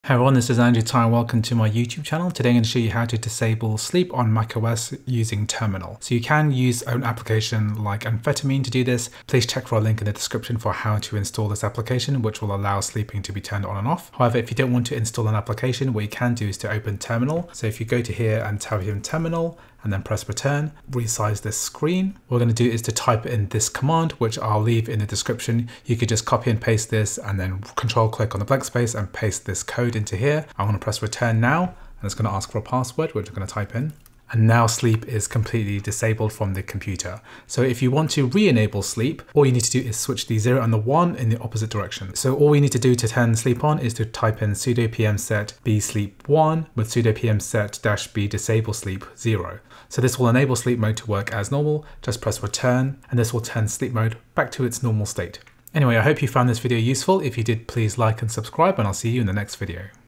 How To Stop Mac Sleeping Using Terminal Commands
Turn off sleep:
sudo pmset -a sleep 0; sudo pmset -a hibernatemode 0; sudo pmset -a disablesleep 1

Turn on sleep:
sudo pmset -a sleep 1; sudo pmset -a hibernatemode 0; sudo pmset -a disablesleep 0

An alternative is to use the System Preferences settings or to install a free application like Amphetamine, see this tutorial